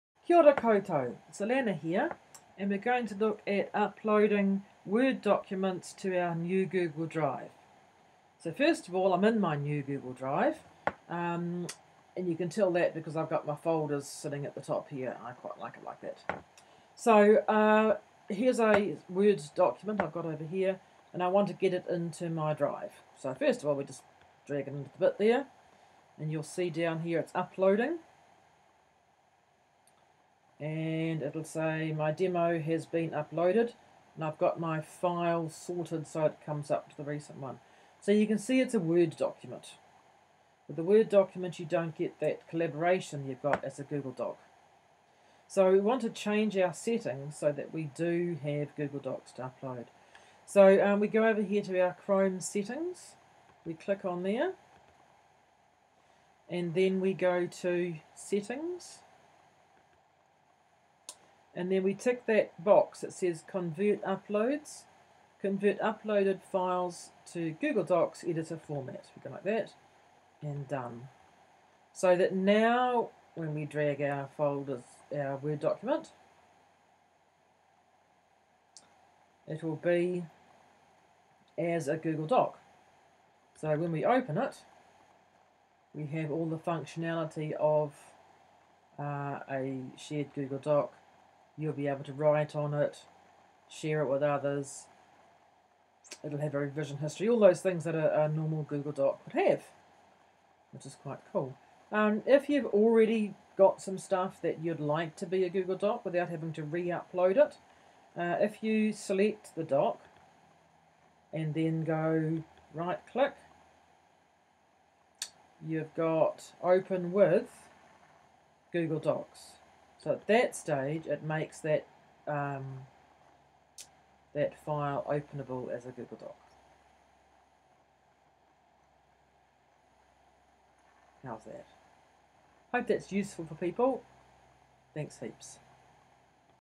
Saving Word Files as Google Docs
Chrome Drive Settings Cogwheel- Settings- tick the box that says convert Word Docs to Google Docs- Save.